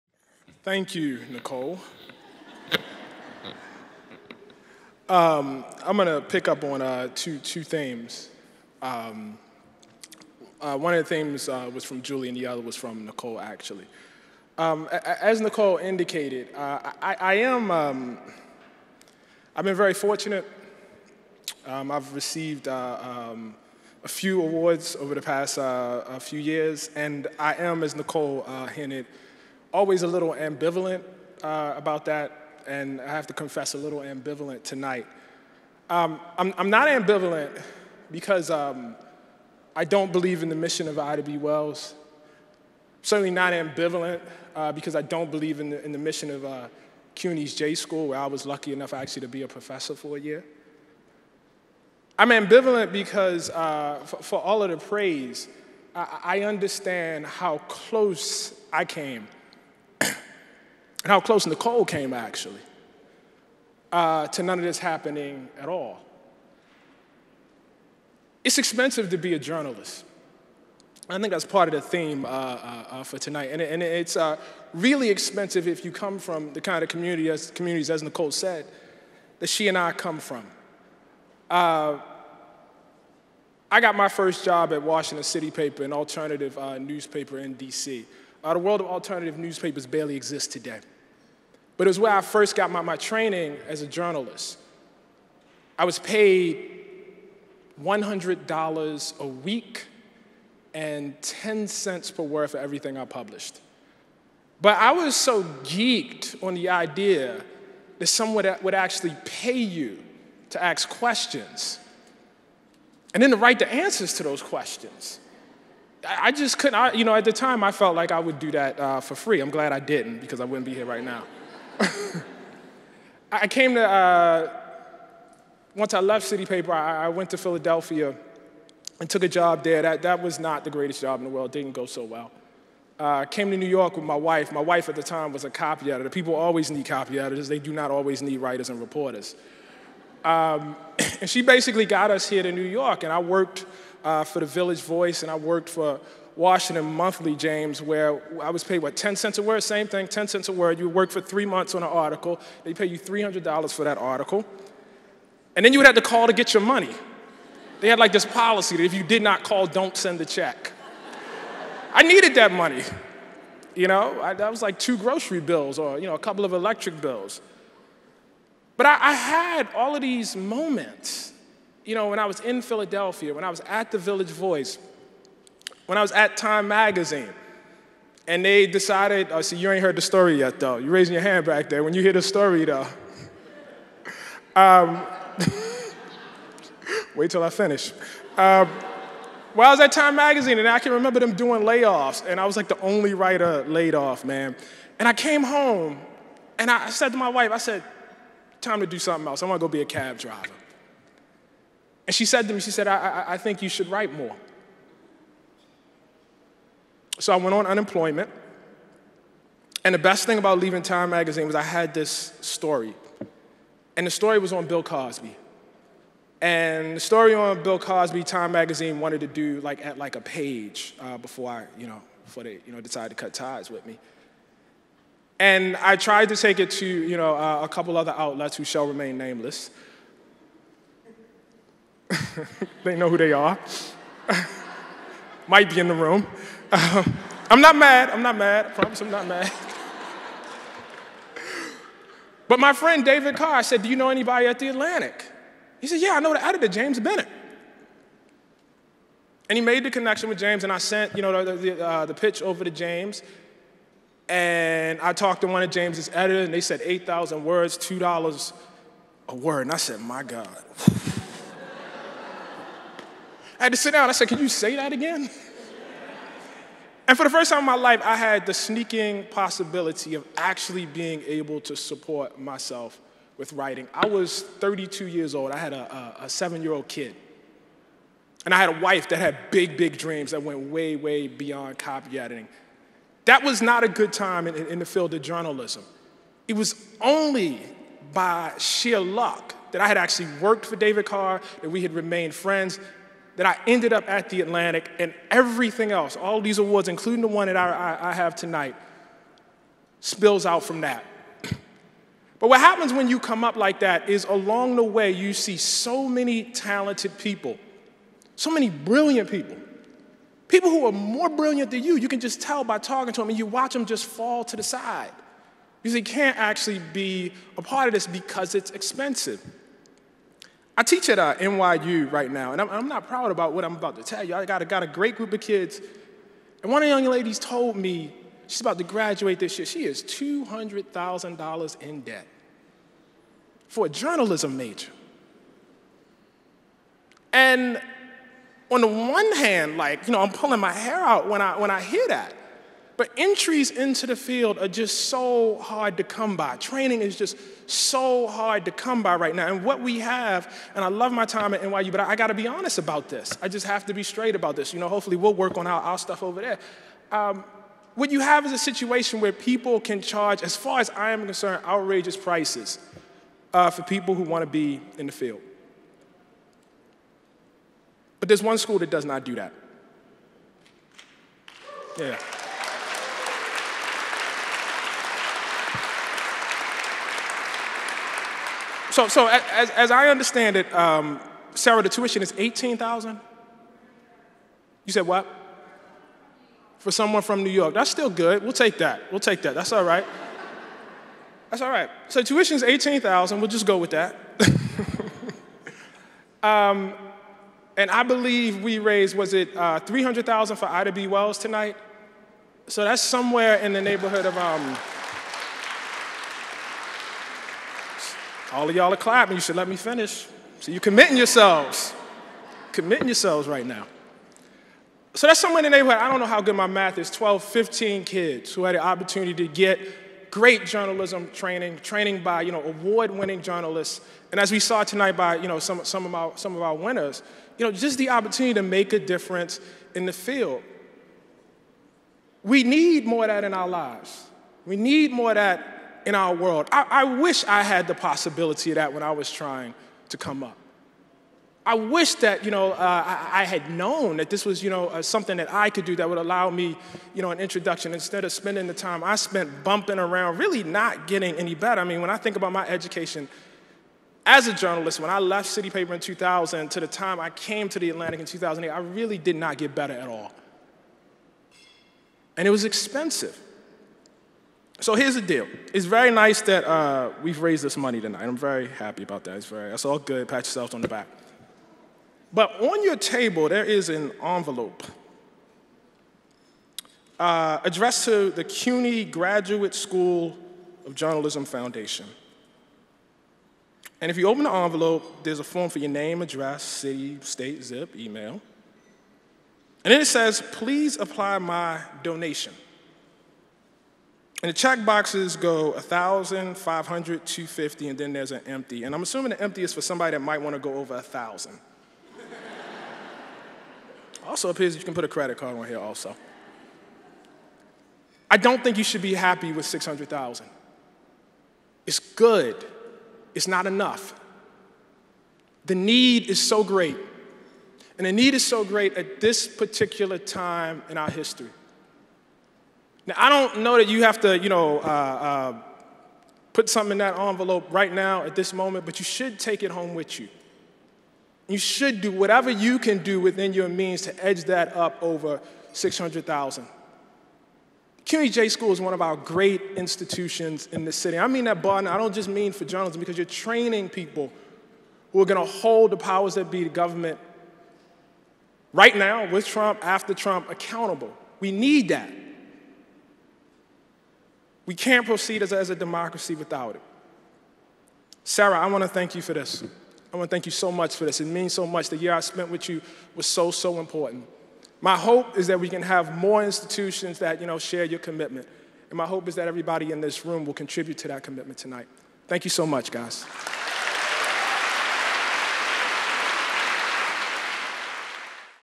Ta-Nehisi Coates Accepts 2018 Journalistic Achievement Award
Ta-Nehisi Coates, National Correspondent for The Atlantic, accepts the 2018 Journalistic Achievement Award, presented on April 11, 2018, at Cipriani 25 Broadway.